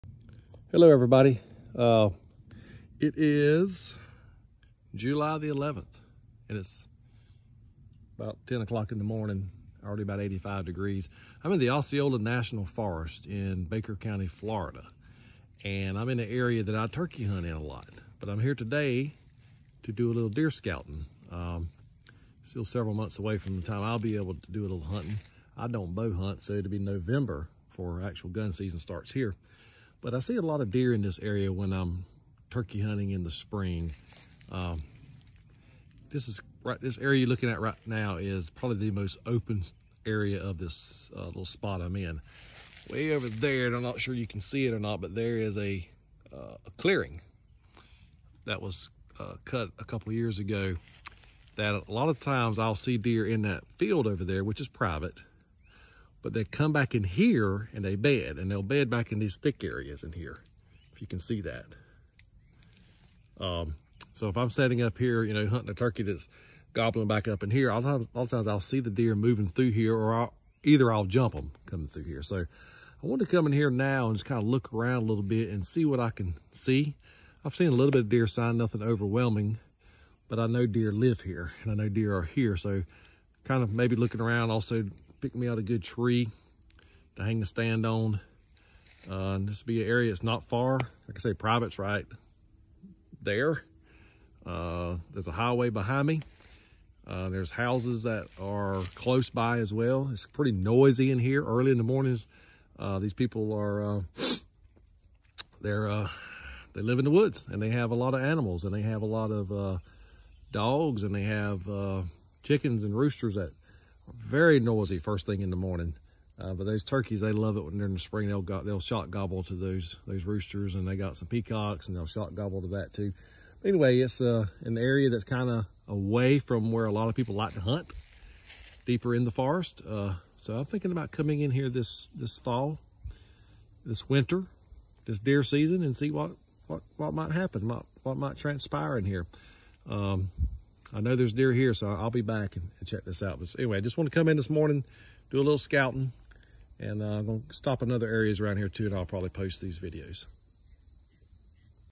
Scouting for deer in the Osceola National Forest
Scouting deer in the summer in North Florida.